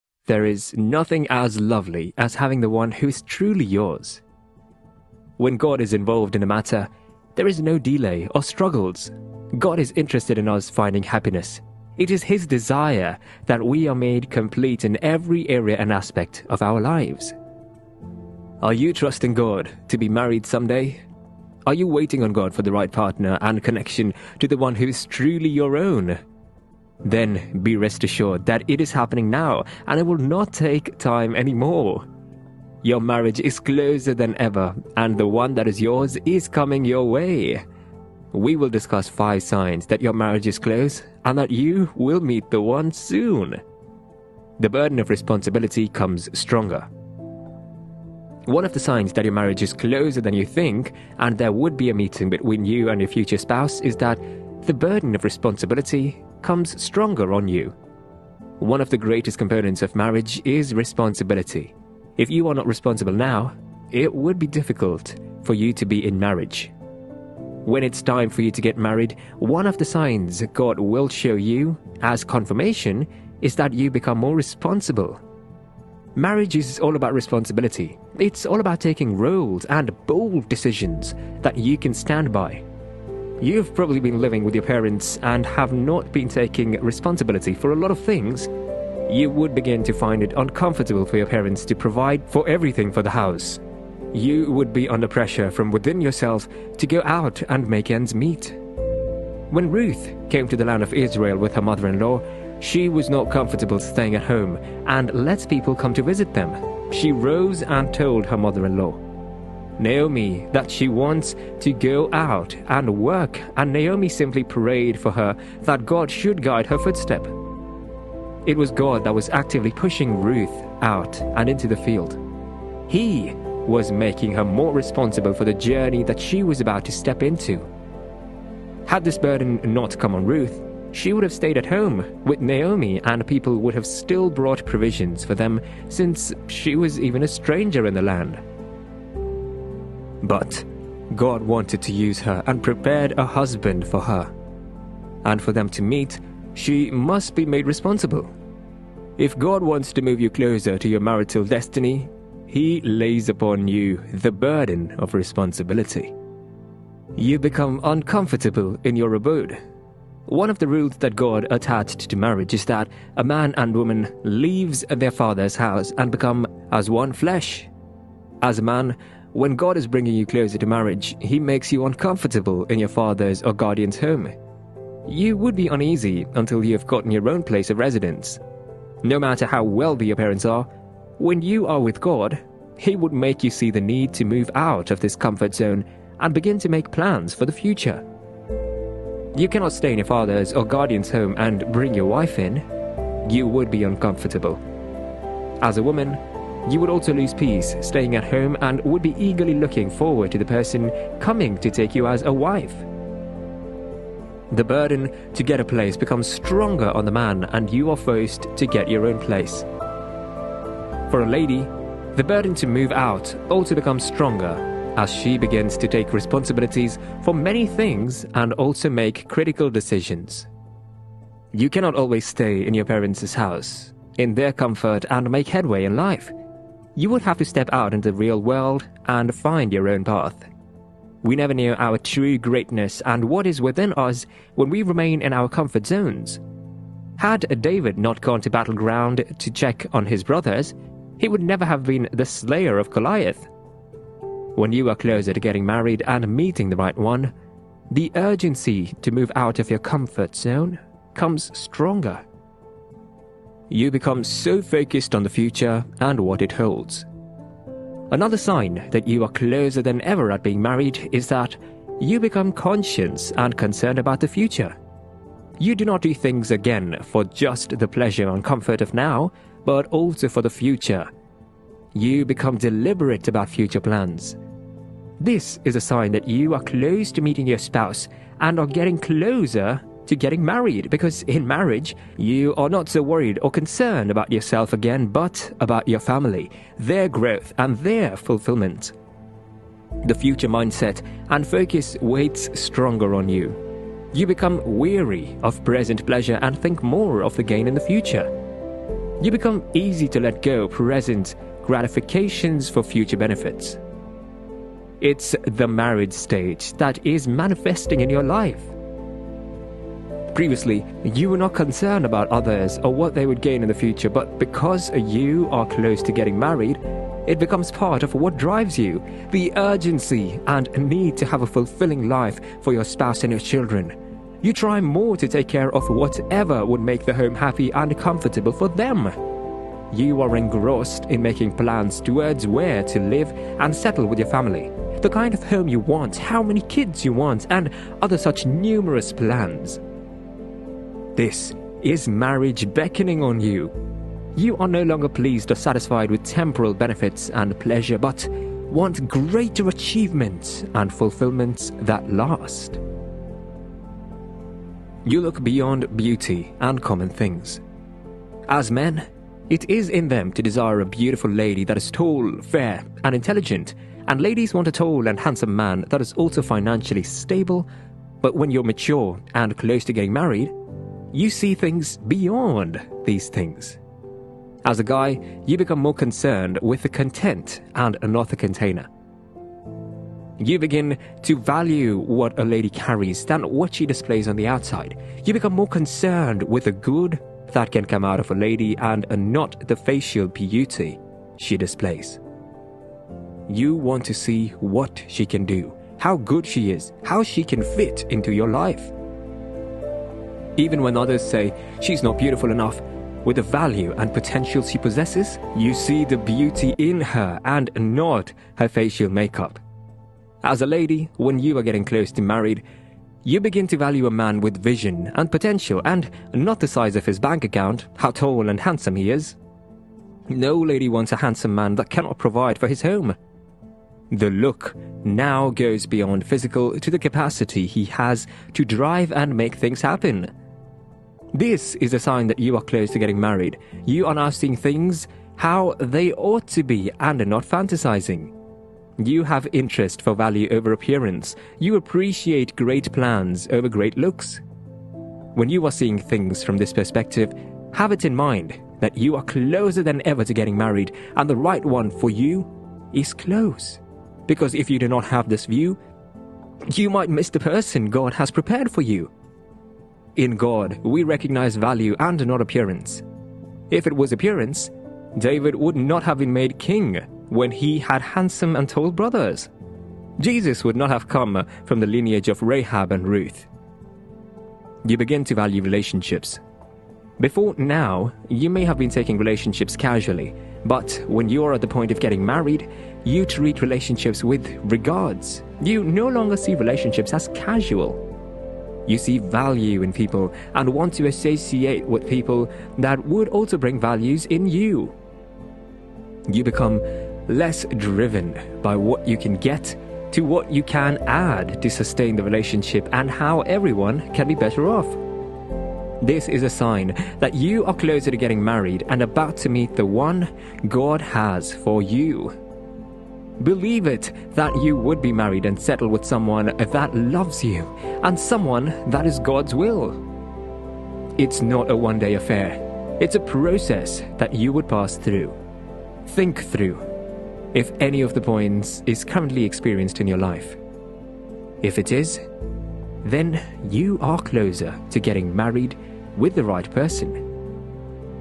5 Signs God is Preparing & Pulling You To Someone You Will Love and be Married To Very SOON
There is nothing as lovely as having the one who is truly yours. When God is involved in a matter, there is no delay or struggles. God is interested in us finding happiness; it is his desire that we are made complete in every area and aspect of our lives. Are you trusting God to be married someday? Are you waiting on God for the right partner and connection to the one who is truly your own? Then be rest assured that it is happening now and it will not take time anymore. Your marriage is closer than ever and the one that is yours is coming your way